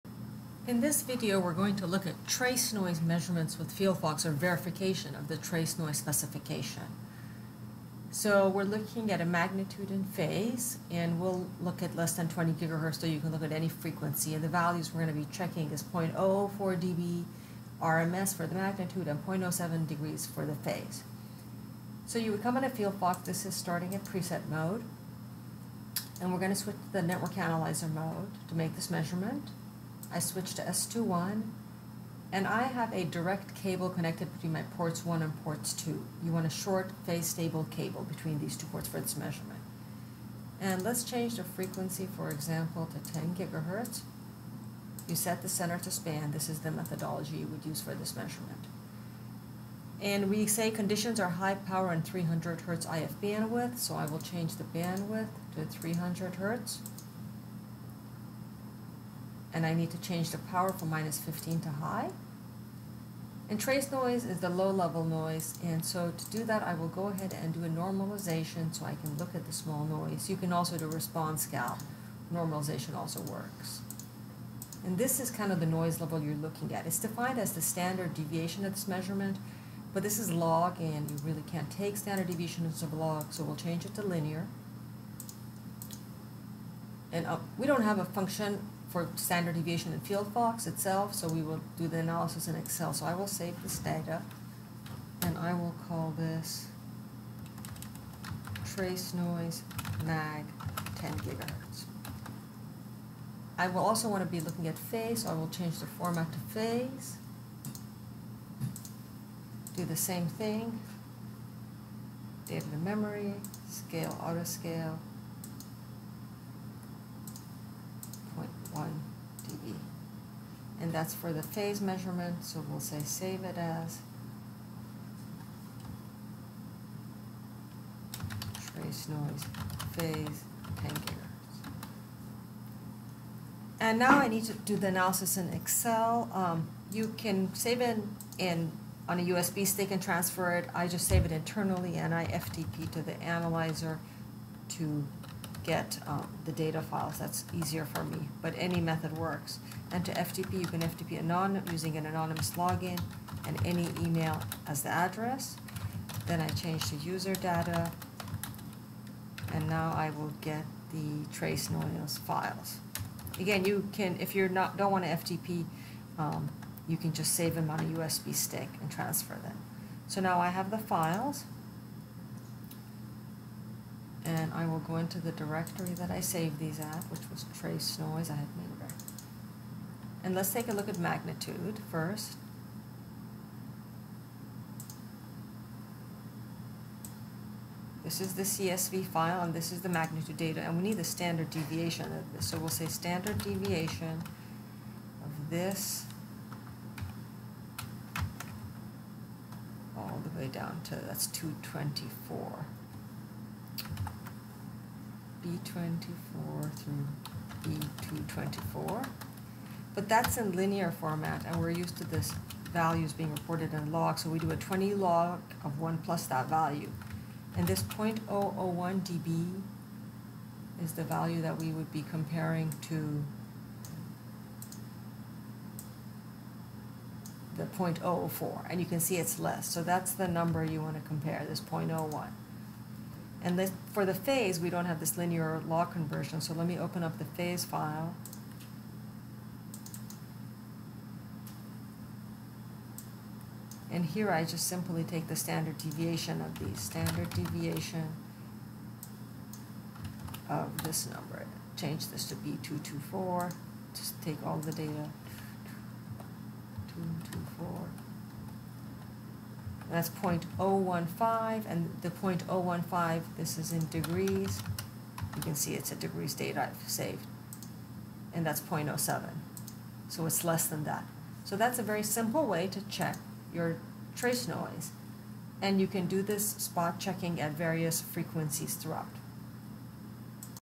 Keysight FieldFox VNA Trace Noise Check
This video shows how to do a quick check of the trace noise (mag and phase) of FieldFox. The instrument should be on for at least two hours prior to the test to ensure ultimate stability.